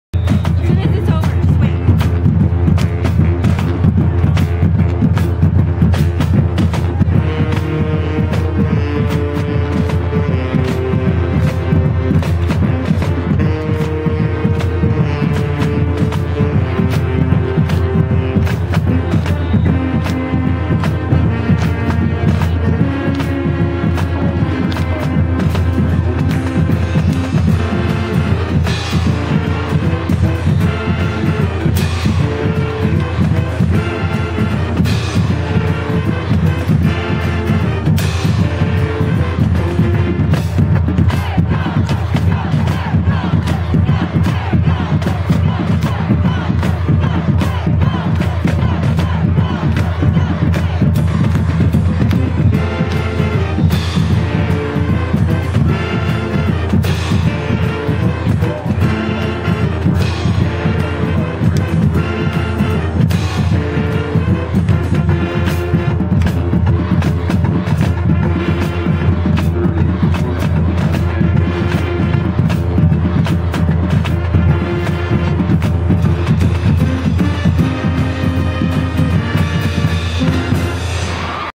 The John Jay Mustang Band performs at the Clark game, October 17, 2013.